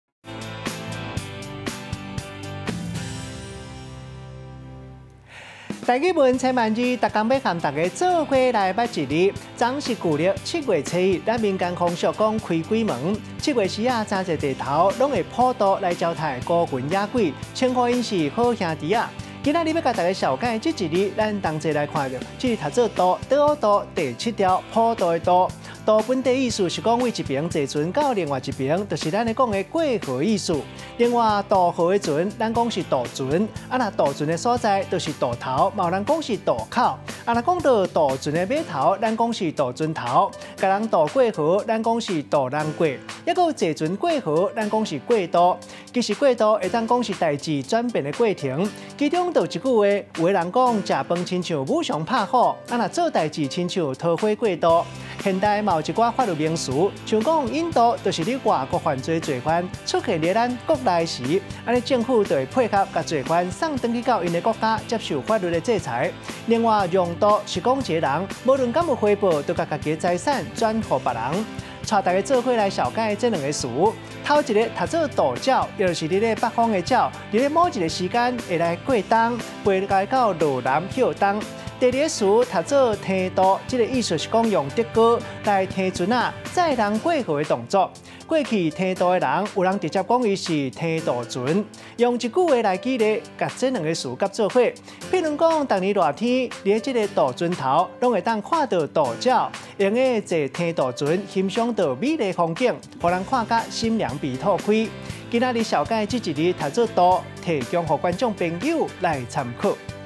2024.8.5.台視台語新聞主播郭于中逐工一字「渡」（tōo）
臺語文千萬字，
逐工欲和逐家做伙來捌一字。
昨昏是舊曆七月初一，
民間風俗「開鬼門」，
七月時仔誠濟地頭攏會「普渡」
（按，古早臺日寫「普度」）
招待孤魂野鬼，
稱呼怹是「好兄弟仔」。
今仔日欲共逐家紹介這字，
「渡」（tōo）。

「渡」（tōo）。
第七調。
「渡」是「對一爿坐船到另外一爿。」
也就是「過河」的意思；
渡河的船，
咱講是「渡船」；
渡船的所在，是「渡頭」，
嘛有人講「渡口」；

渡河船的碼頭，
咱講是「渡船頭」；
共人渡過河，是「渡人過」。
坐船過河，
是「過渡」，
嘛會用得講是代誌轉變的過程。
有一句話講，有的人「食飯像武松拍虎，
做代誌親像桃花過渡。」
現代有一寡法律的名詞，像「引渡」，
是佇外國犯罪的罪犯，
出現佇咱國內的時，
政府配合共罪犯送轉去怹的國家，
接受法律制裁。
另外「讓渡」，是講一e人，
無論敢有回報，
就共家己的財轉，
轉予別人。

《唐韻》《集韻》《韻會》
徒故切
《正韻》獨故切，音度。
《說文》濟也。
《廣雅》去也，通也。又通作度。
《集韻》或作𣳥。

（The meaning of word is to cross or ferry over the river.）

做伙來紹介兩ê詞：
第一ê詞：渡鳥（tōo-tsiáu），
就是佇北方的鳥，
佇某一ê時間為著欲過冬會飛落南來歇冬。。

第二ê詞：撐渡（the-tōo），
意思是用竹篙撐船載人過河這ê動作。
過去撐渡的人，

有人直接講伊是「撐渡船」。

咱用一句話來舉例，
共這兩ê詞佮做伙。
譬論講：
逐年熱天佇這ê渡船頭，
會當看著渡鳥，
閣會當坐撐渡船，
欣賞著美麗的風景，
予人心涼脾土開。

參考資料：
《教育部臺灣閩南語常用詞辭典》